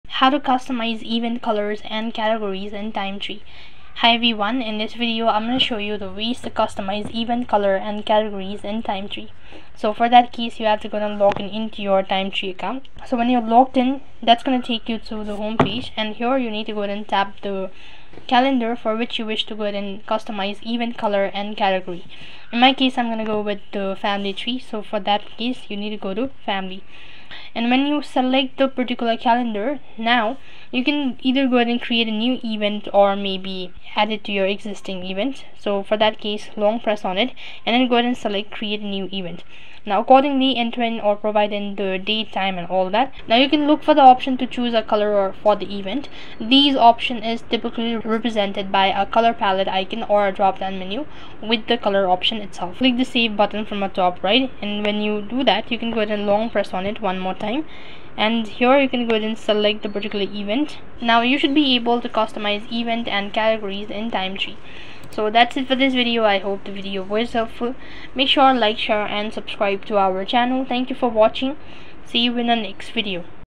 How To Customize Event Colors And Categories In Timetree !
This Video Is based on How you can Customize Event Colors And Categories In Timetree.

Chapters / Timestamps
0:00 Introd
0:11 Customize Event Colors And Categories In Timetree
1:06 Outro